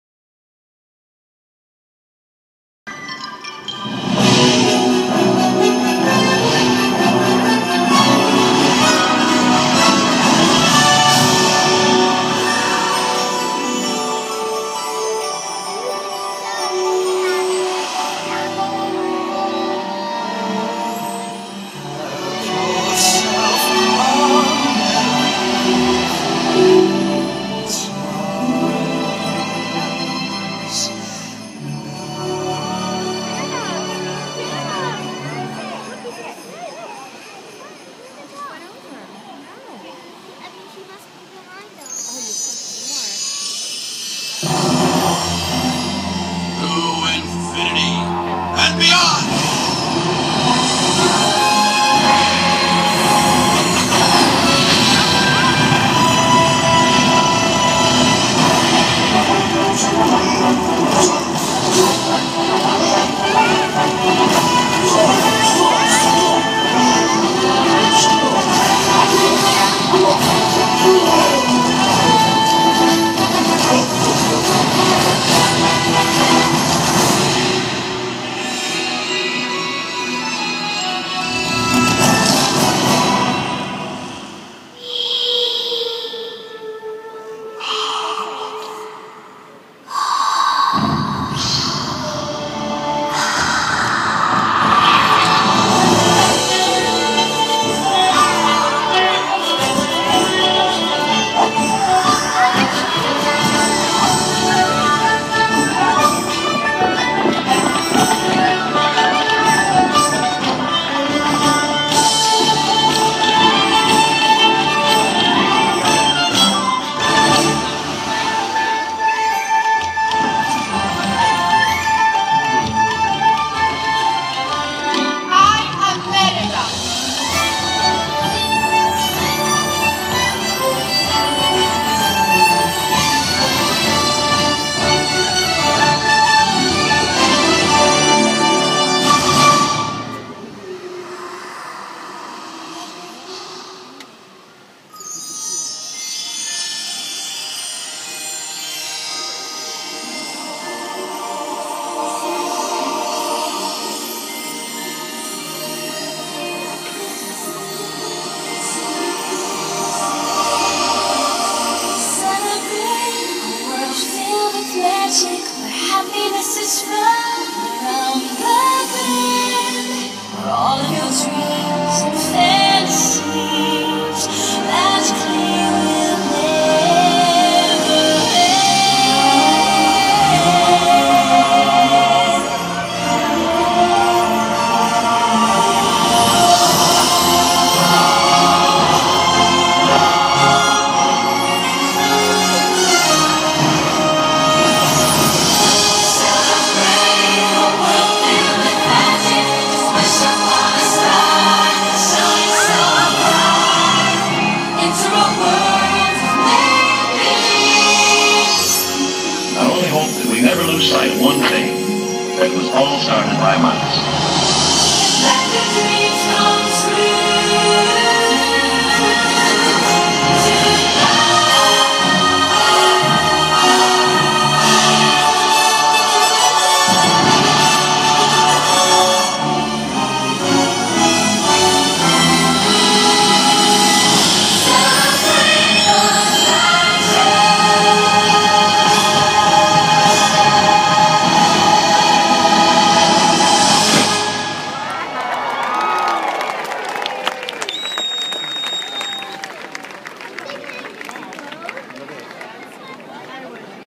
Walt Disney Castle 2012
See the wonderful colorful change of the Walt Disney Castle. Recorded on 11/15/2012
Original sound from that day. (Copyright Walt Disney)